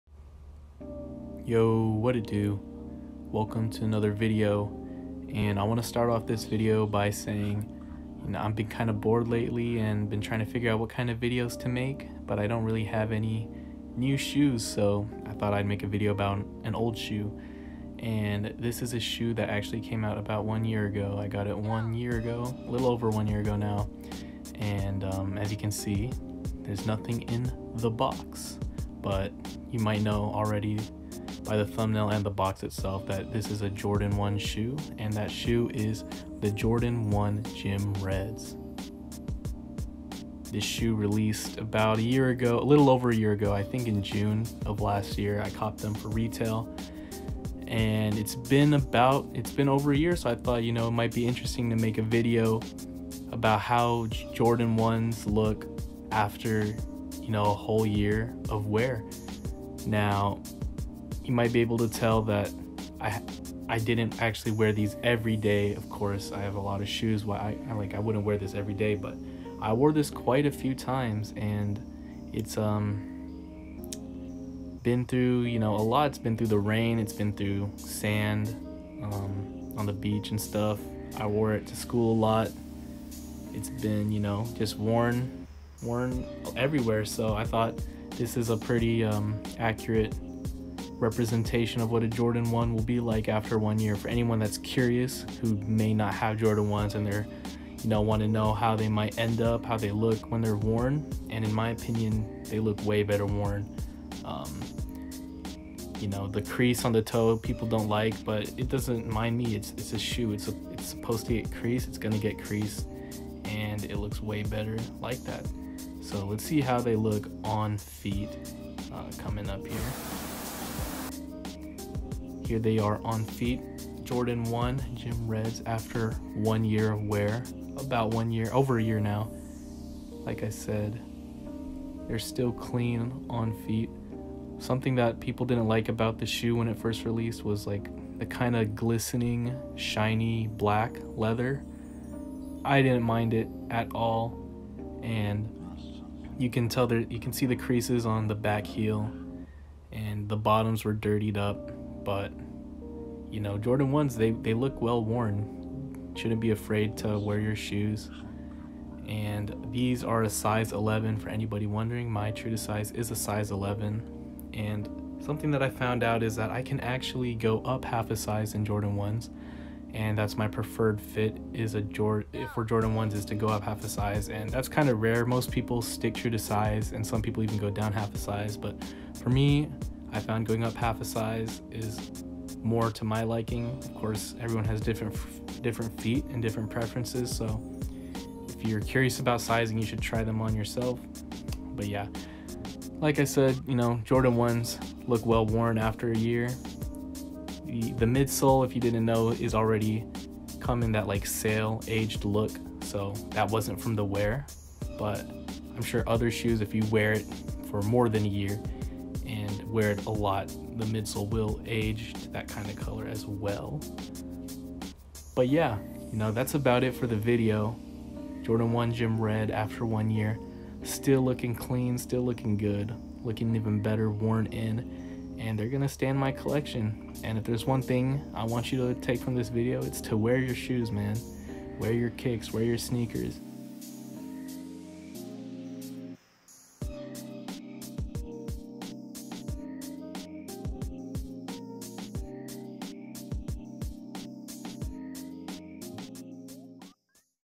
JORDAN 1 HIGH AFTER 1 YEAR OF WEAR! w/ On Feet
Yooo! This video is more meant for people who don’t have Jordan 1s and are curious to see how they look after wear. Today, we take a look at a pair of shoes I’ve been rocking for a little over a year now, and that’s the Jordan 1 High Gym Reds. I wanted to make a video, but I don’t have any new shoes at the moment, so I’m turning towards my older pairs. I hope you enjoy!